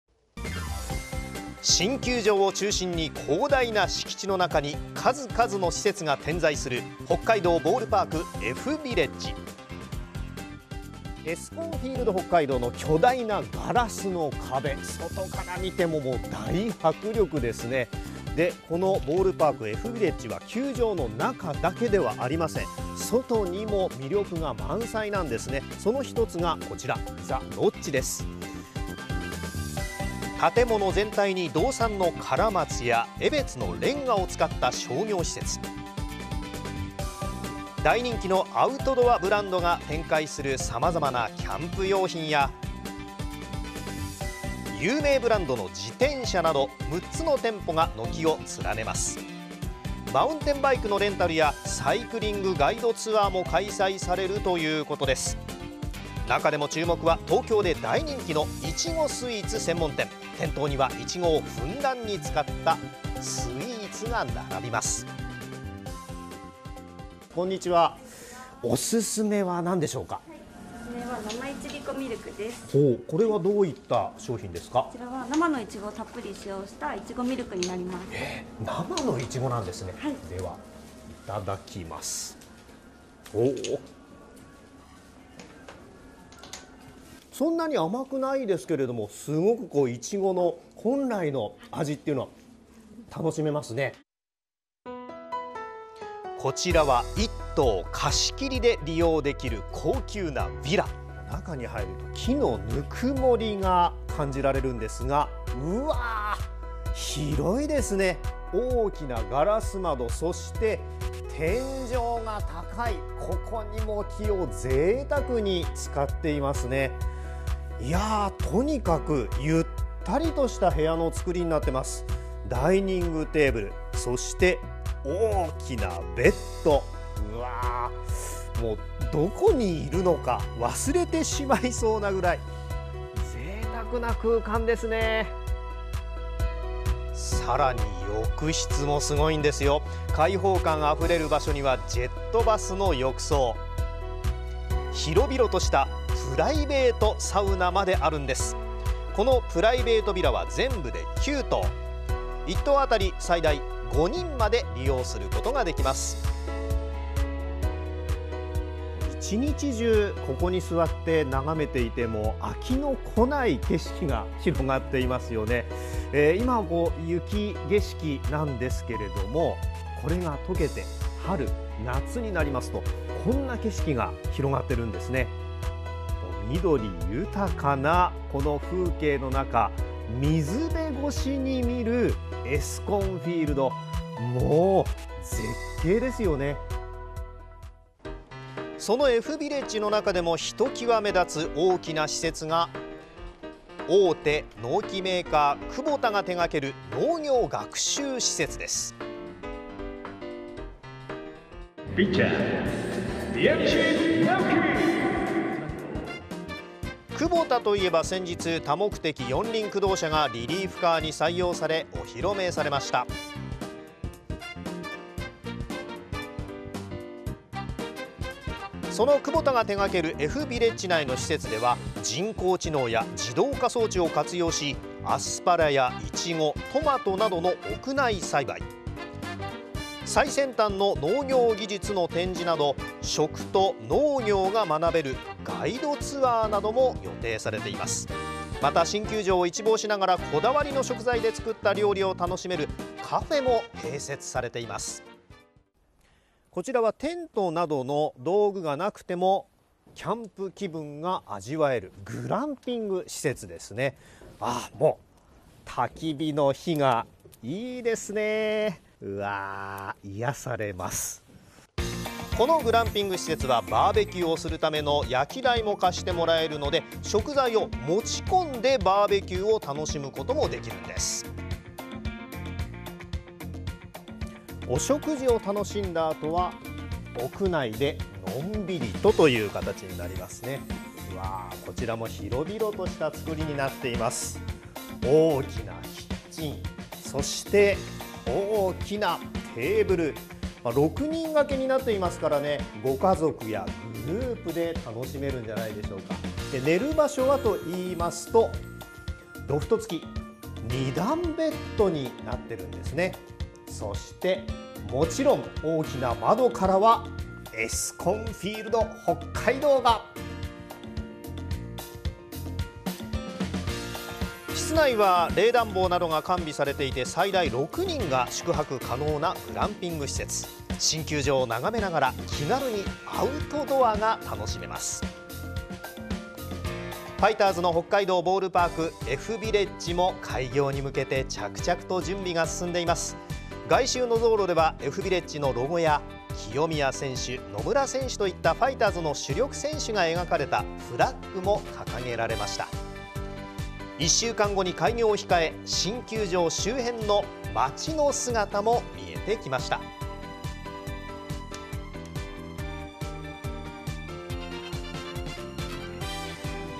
【球場周辺も魅力満載】まるでテーマパーク 新施設が続々 ボールパーク大公開
ファイターズの新たな本拠地、北海道ボールパークＦビレッジの開業まで１週間。野球だけではない魅力がたっぷりのいわゆるテーマパークです。これまでの球場にはなかった施設の数々取材しました。（2023年3月7日放送）